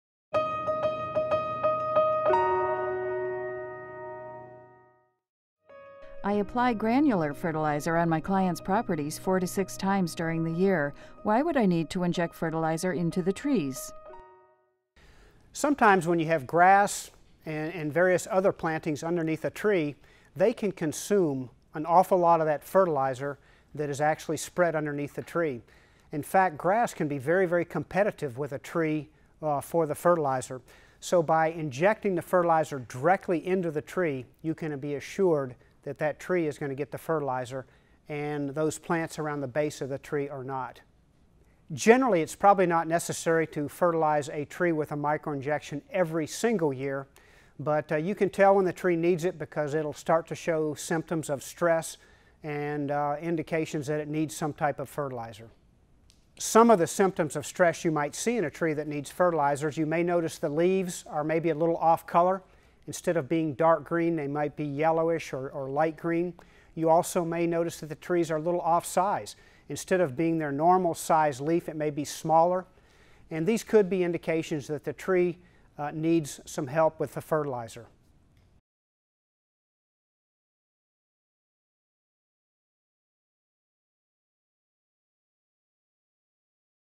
Why would I need to inject fertilizer into trees?
I apply granular fertilizer on my clients’ properties 4-6 times during the year.
Why would I need to inject fertilizer into the trees?